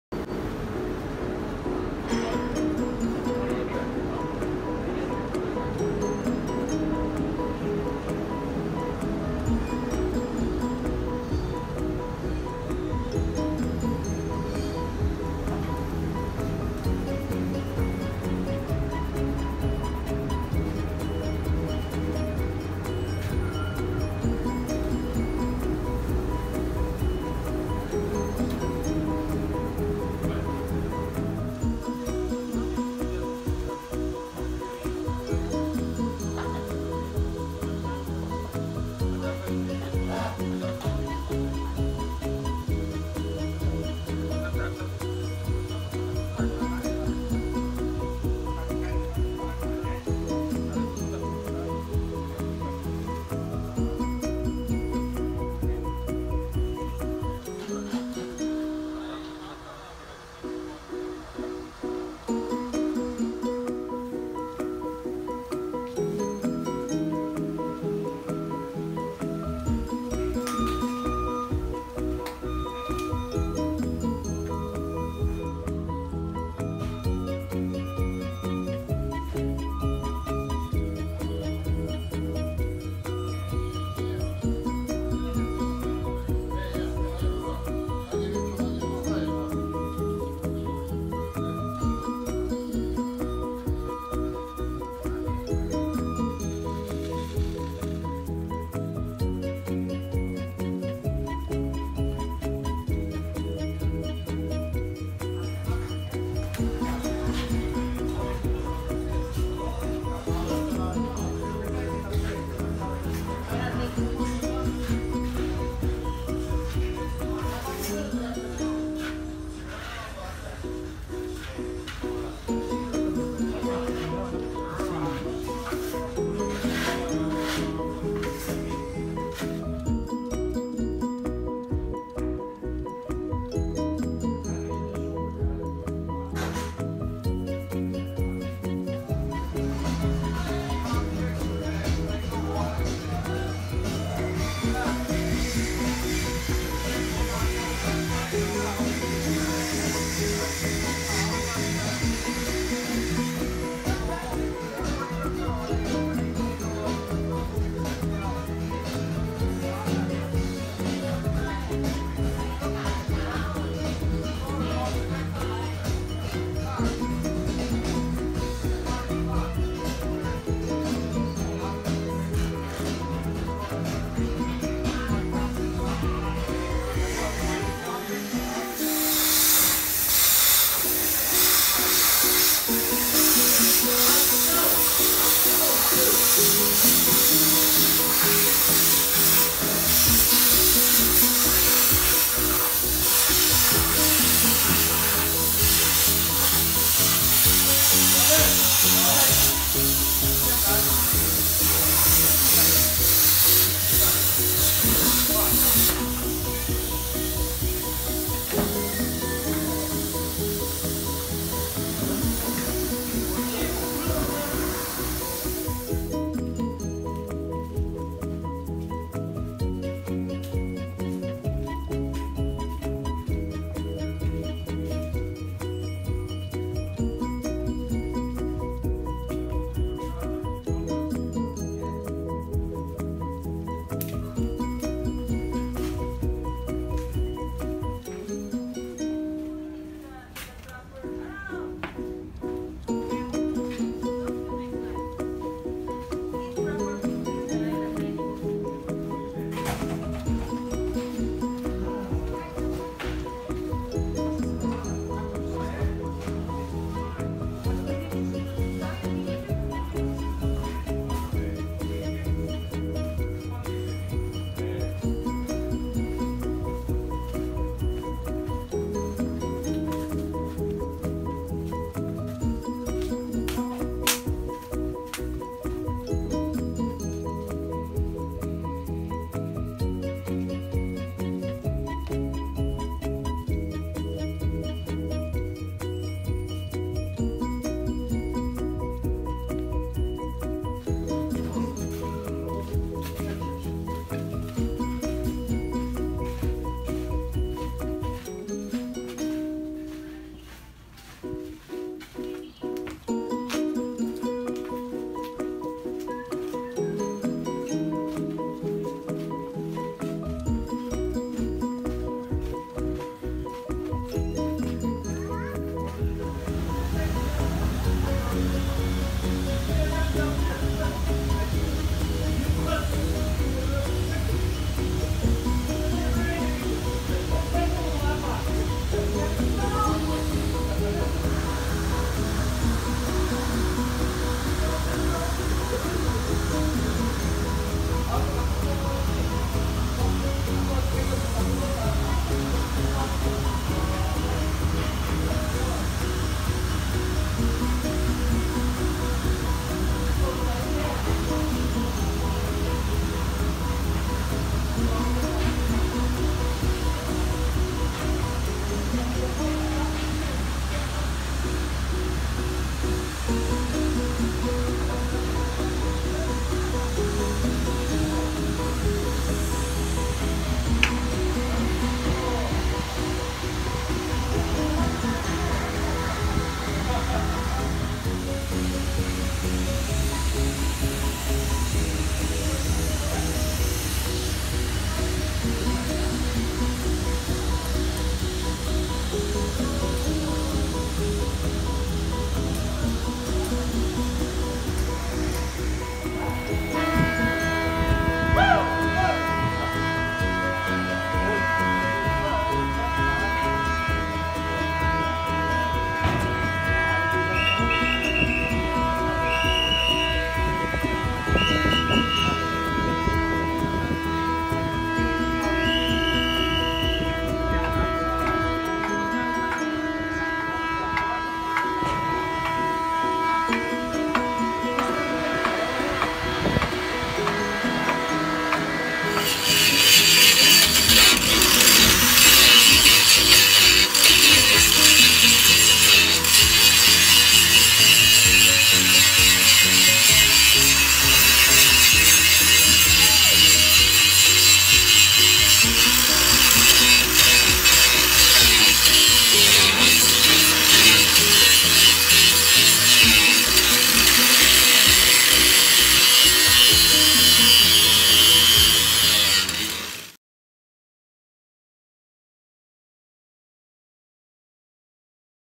Cruise ship dry dock upgraded to higher standard
man at work to level the more fantastic wonderful cruising experience at sea.